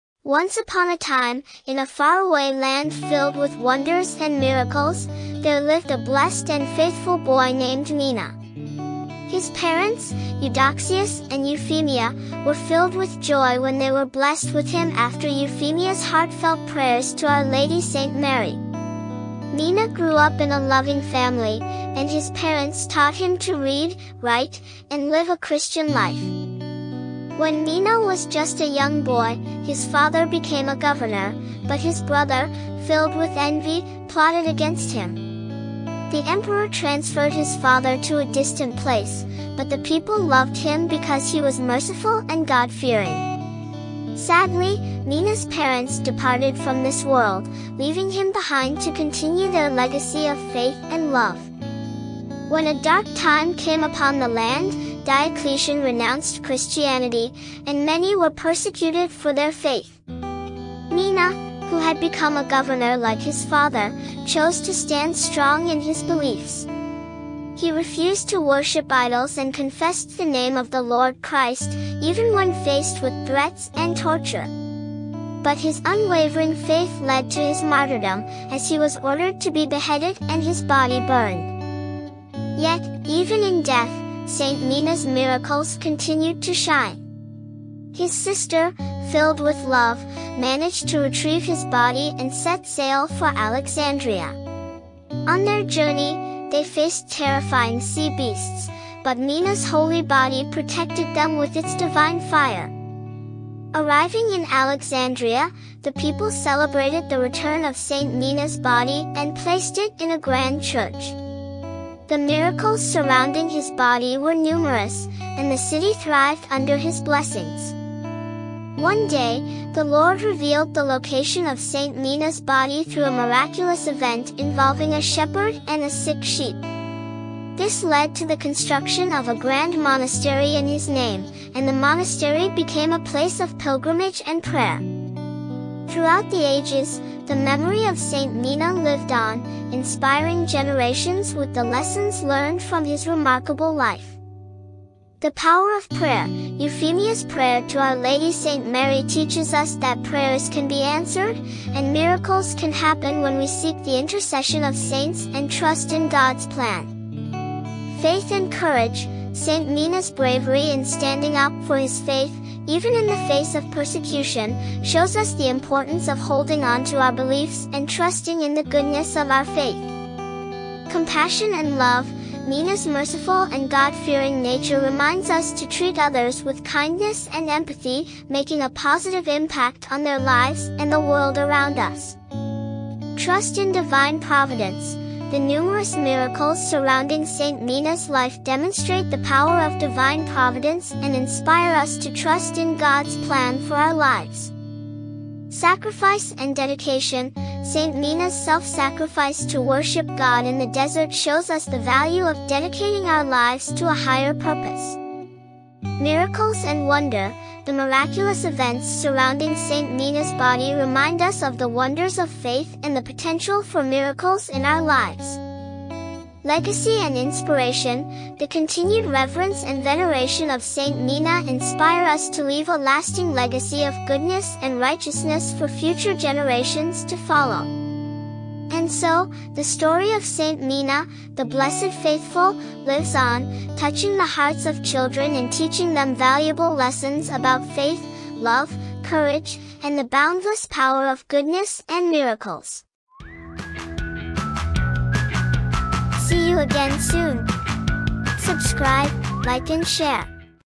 St. Mina
Once upon a time, in a faraway land filled with wonders and miracles, there lived a blessed and faithful boy named Mina. His parents, Eudoxius and Euphemia, were filled with joy when they were blessed with him after Euphemia's heartfelt prayers to Our Lady St. Mary. Mina grew up in a loving family, and his parents taught him to read, write, and live a Christian life.
When Mina was just a young boy, his father became a Governor, but his brother, filled with envy, plotted against him. The Emperor transferred his father to a distant place, but the people loved him because he was merciful and God-fearing. Sadly, Mina's parents departed from this world, leaving him behind to continue their legacy of faith and love.
When a dark time came upon the land, Diocletian renounced Christianity, and many were persecuted for their faith. Mina, who had become a Governor like his father, chose to stand strong in his beliefs. He refused to worship idols and confessed the Name of the Lord Christ, even when faced with threats and torture. But his unwavering faith led to his martyrdom, as he was ordered to be beheaded and his body burned.
Yet, even in death, St. Mina's miracles continued to shine. His sister, filled with love, managed to retrieve his body and set sail for Alexandria. On their journey, they faced terrifying sea beasts, but Mina's holy body protected them with its divine fire.
Arriving in Alexandria, the people celebrated the return of St. Mina's body and placed it in a grand church. The miracles surrounding his body were numerous, and the city thrived under his blessings.
One day, the Lord revealed the location of St. Mina's body through a miraculous event involving a shepherd and a sick sheep. This led to the construction of a grand monastery in his name, and the monastery became a place of pilgrimage and prayer.
Throughout the ages, the memory of St. Mina lived on, inspiring generations with the lessons learned from his remarkable life:
The Power of Prayer: Euphemia's prayer to Our Lady St. Mary teaches us that prayers can be answered, and miracles can happen when we seek the intercession of saints and trust in God's plan.
Faith and Courage: St. Mina's bravery in standing up for his faith, even in the face of persecution, shows us the importance of holding onto our beliefs and trusting in the goodness of our faith.
Compassion and Love: Mina's merciful and God-fearing nature reminds us to treat others with kindness and empathy, making a positive impact on their lives and the world around us.
Trust in Divine Providence: The numerous miracles surrounding St. Mina's life demonstrate the power of divine providence and inspire us to trust in God's plan for our lives.
Sacrifice and Dedication: St. Mina's self-sacrifice to worship God in the desert shows us the value of dedicating our lives to a higher purpose.
Miracles and Wonder: The miraculous events surrounding St. Mina's body remind us of the wonders of faith and the potential for miracles in our lives.
Legacy and Inspiration: The continued reverence and veneration of St. Mina inspire us to leave a lasting legacy of goodness and righteousness for future generations to follow.
And so, the story of St. Mina, the blessed faithful, lives on, touching the hearts of children and teaching them valuable lessons about faith, love, courage, and the boundless power of goodness and miracles.